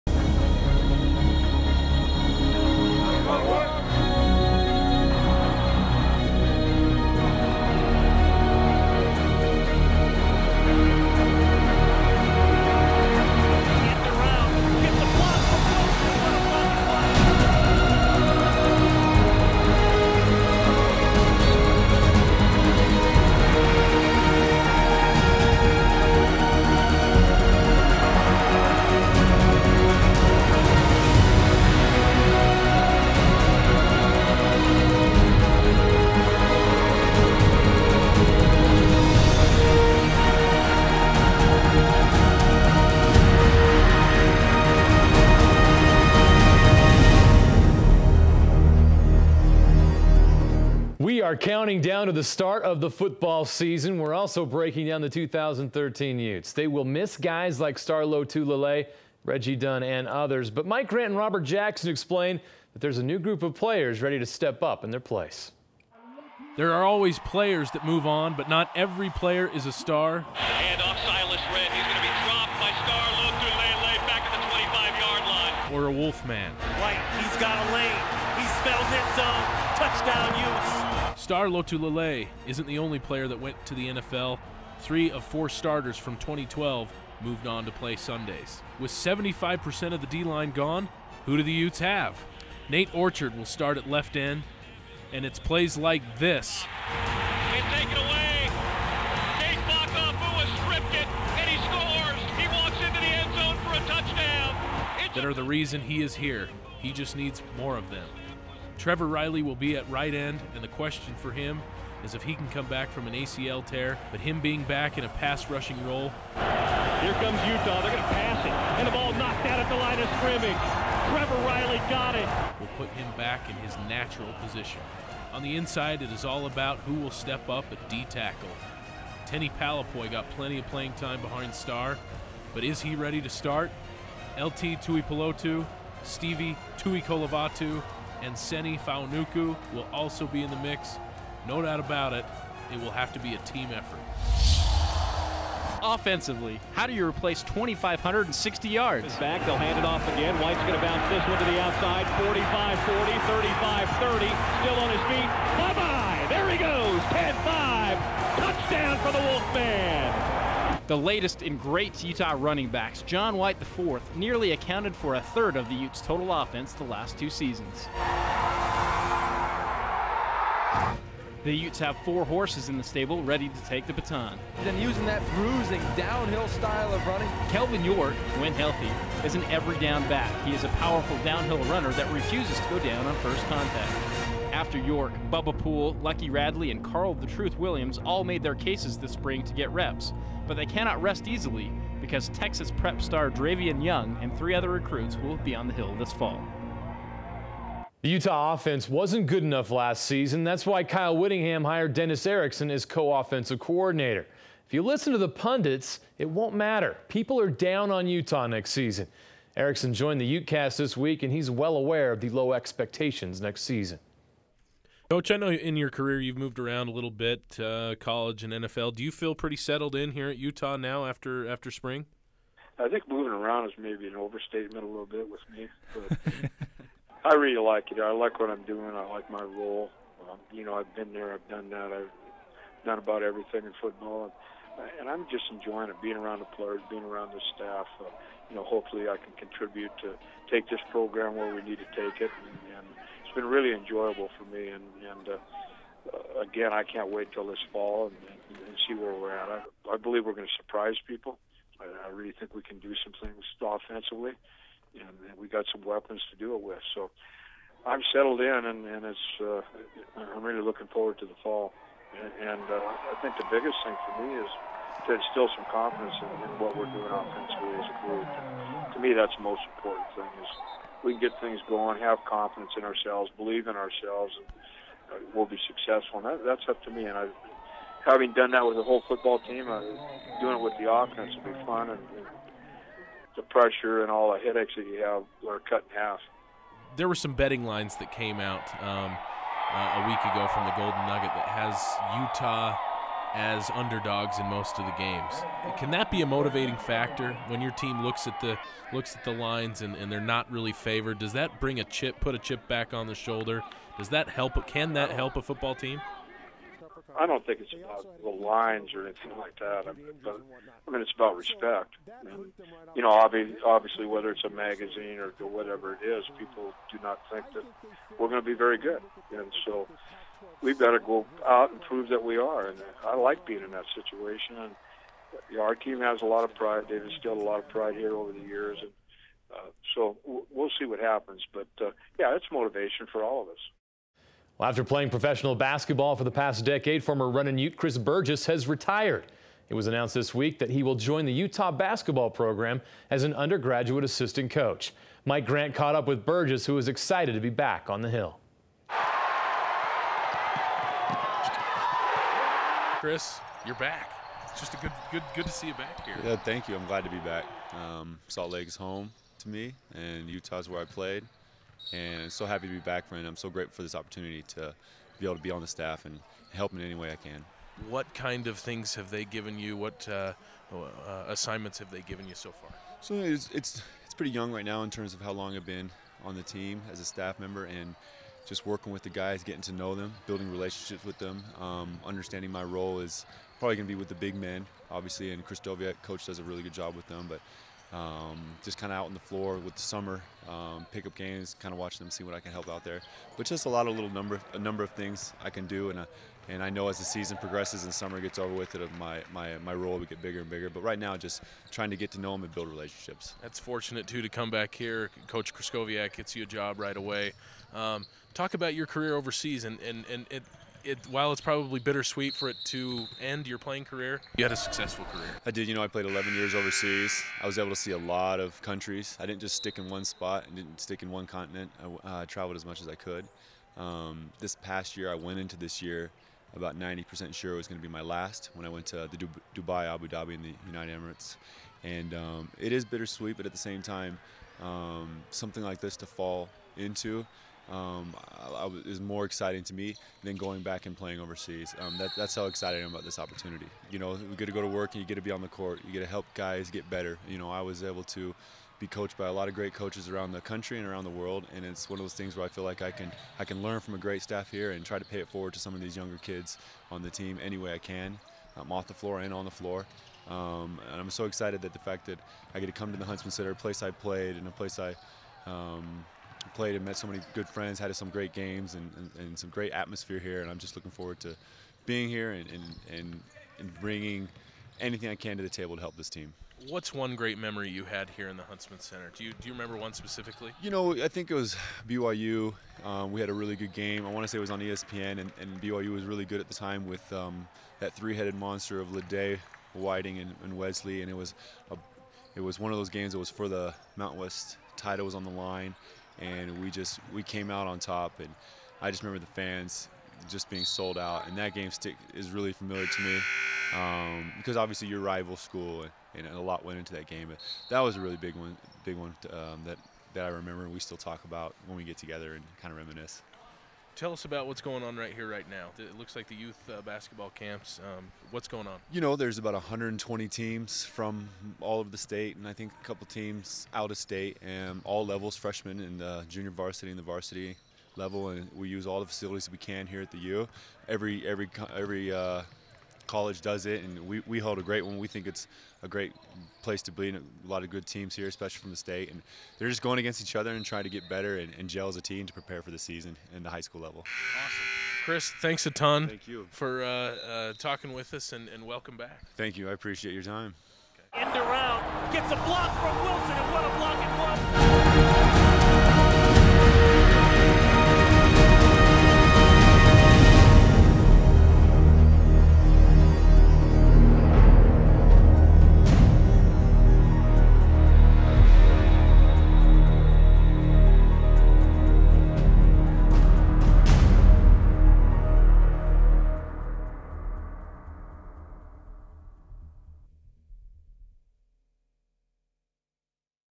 Crimson Corner June 16, 2013
This week on the Crimson Corner we explore the Utes biggest needs to replace on the football team, Dennis Erickson was on the UTEcast and knows the Utes aren't getting any respect going into the 2013 season, and we catch up with Chris Burgess who was hired on Larry Krystkowiak's staff.